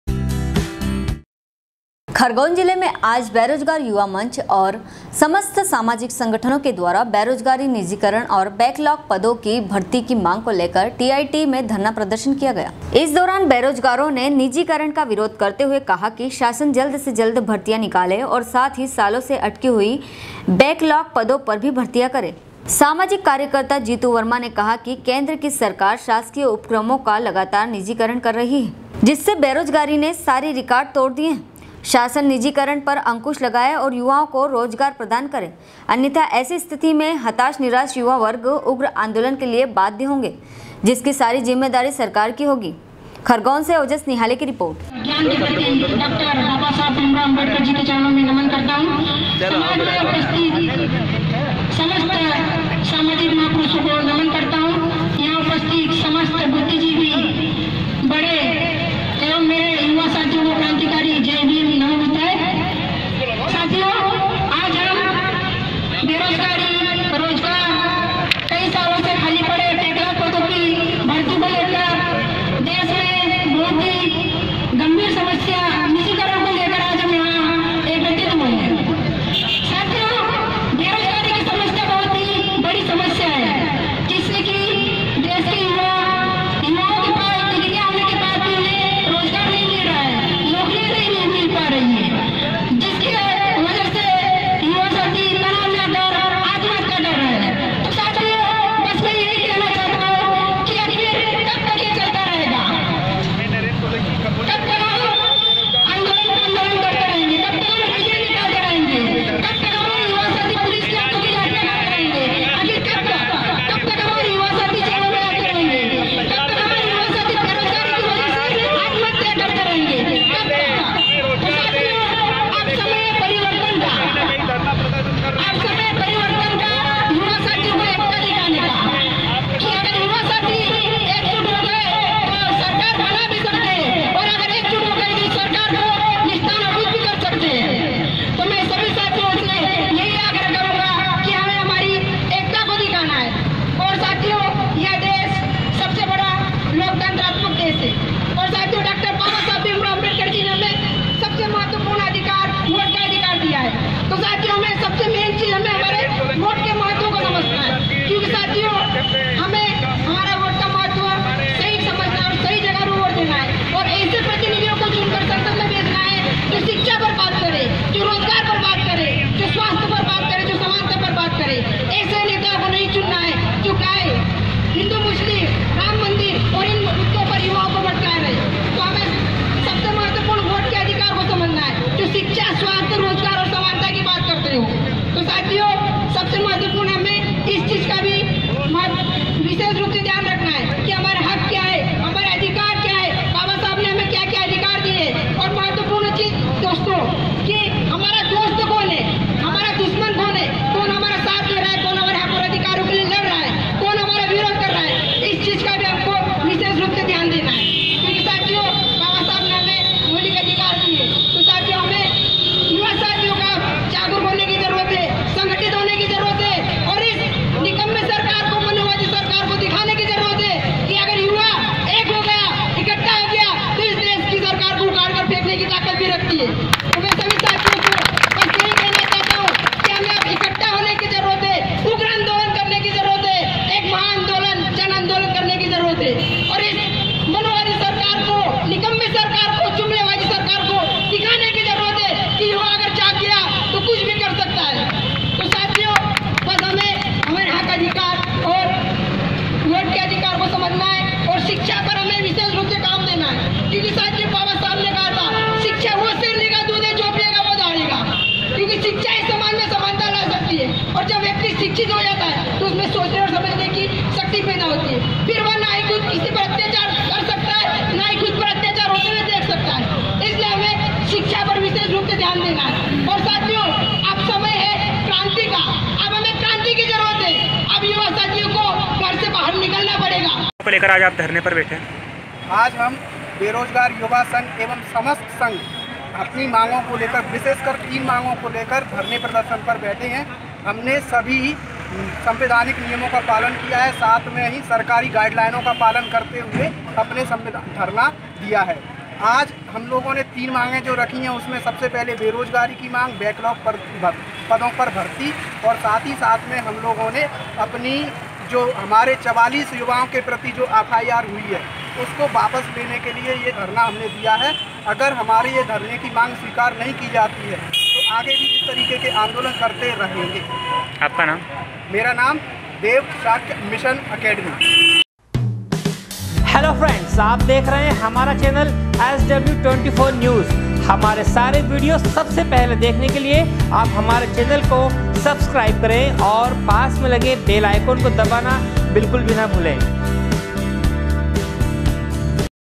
SW 24 News Madhyapradesh
खरगोन जिले में आज बेरोजगार युवा मंच और समस्त सामाजिक सँगठनो के द्वारा बेरोजगारी-निजीकरण और बैकलॉग पदों की भर्ती की मांग को लेकर टीआईटी में धरना प्रदर्शन किया गया। खरगोन से ओजस निहाले की रिपोर्ट